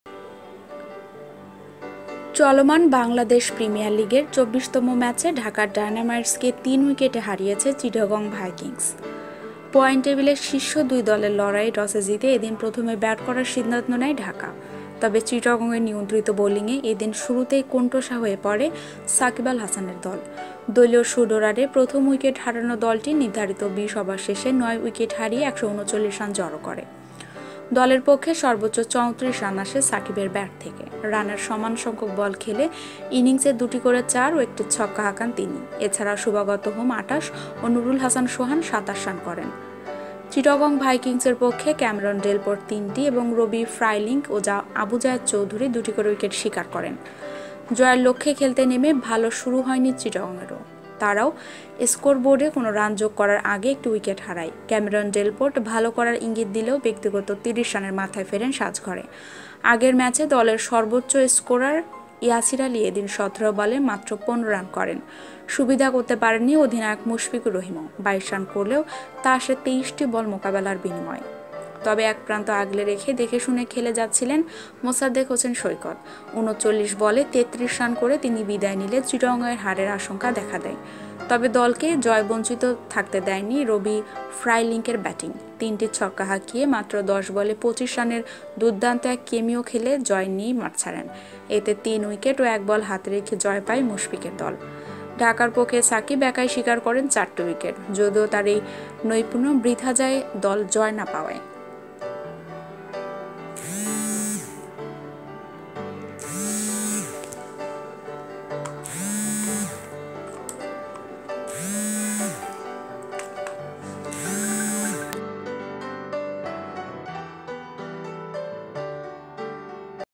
দেখুন.. সহজ লক্ষ্যে নেমেও চিটাগংয়ের কঠিন ও নাটকীয় জয়|chittagong vikings vs dhaka dynamites bpl 2019
We provides bd cricket, bangladesh cricket, bangladesh cricket team, cricket bangladesh, bangladesh cricket news, bangladesh cricket board , bangladesh national cricket team.